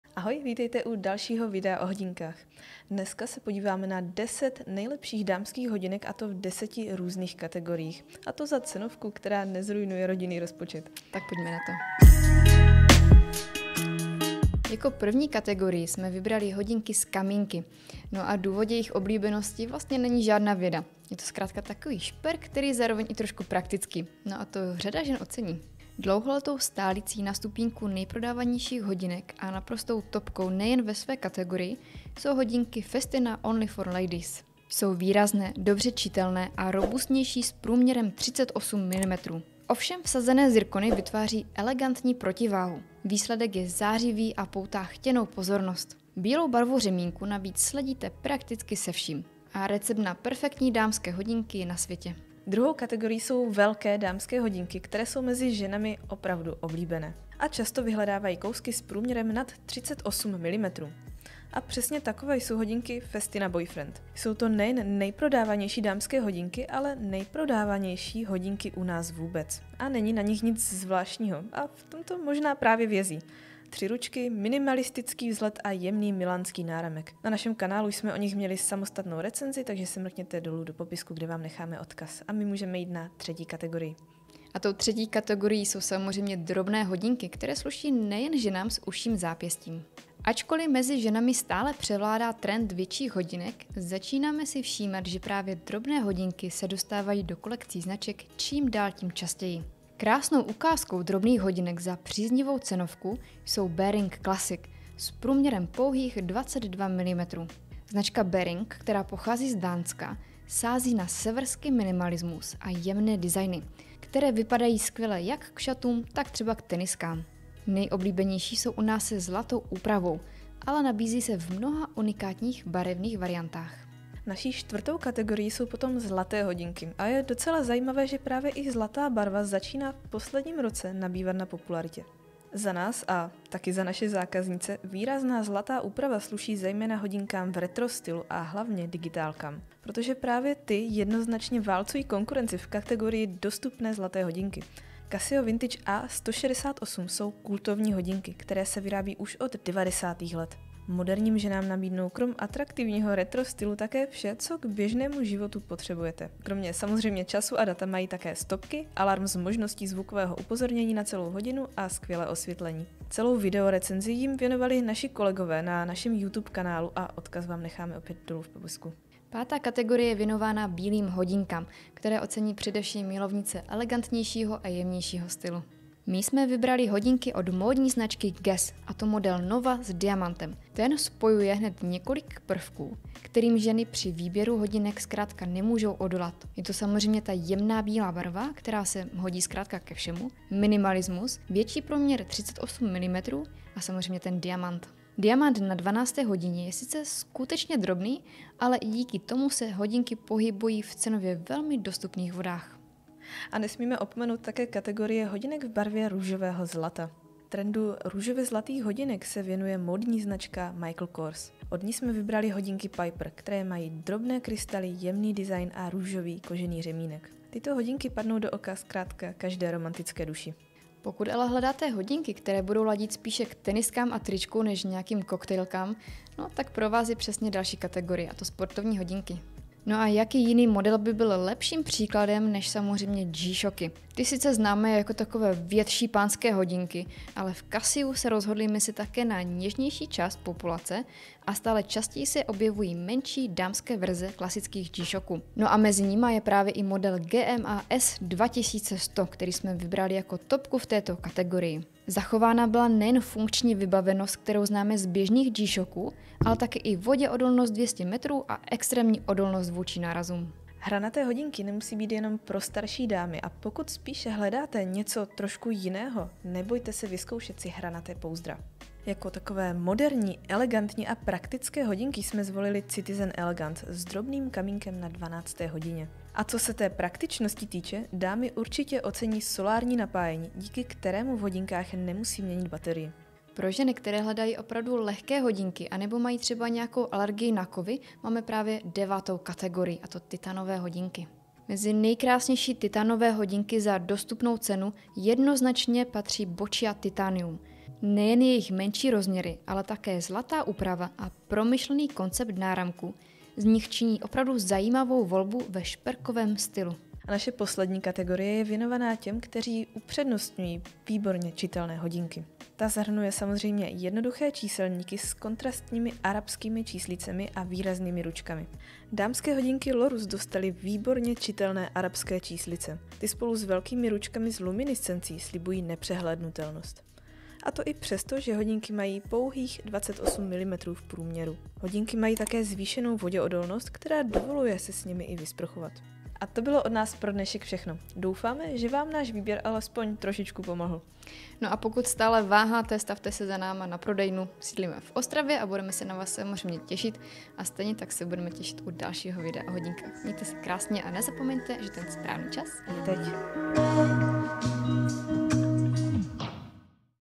Nejlepší dámské hodinky 2023 (cenově dostupné)
NÁŠ E-SHOP

Malé nebo velké? Sportovní nebo elegantní? S diamanty nebo bez? Najít a vybrat zkrátka ty nejkrásnější dámské hodinky může být náročný proces. Pro ty, kteří se stále nemohou rozhodnout a ocenili by kapku inspirace, jsme dnes připravili výběr nejoblíbenějších dámských dostupných hodinek v různých kategoriích.

A nezapomeňte – Ten správný čas je teď.